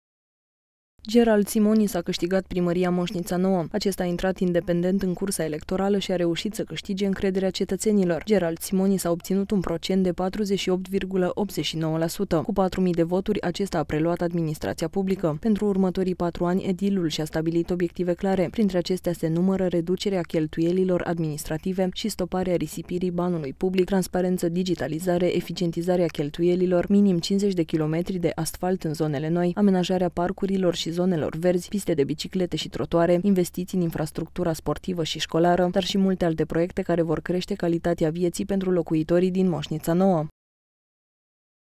Gerald Simonis este primarul comunei Moșnița Nouă
Gerald Simonis a câștigat Primăria Moșnița Nouă. Acesta a intrat independent în cursa electorală și a reușit să câștige încrederea cetățenilor. Gerald Simonis a obținut un procent de 48,89%. Cu 4.000 de voturi, acesta a preluat administrația locală.    Pentru următorii patru ani, edilul și-a stabilit obiective clare. Printre acestea se numără reducerea cheltuielilor administrative și stoparea risipirii banului public, transparență, digitalizare, eficientizarea cheltuielilor, minim 50 de kilometri de asfalt în zonele noi, amenajarea parcurilor și zonelor verzi, piste de biciclete și trotuare, investiții în infrastructura sportivă și școlară, dar și multe alte proiecte care vor crește calitatea vieții pentru locuitorii din Moșnița Nouă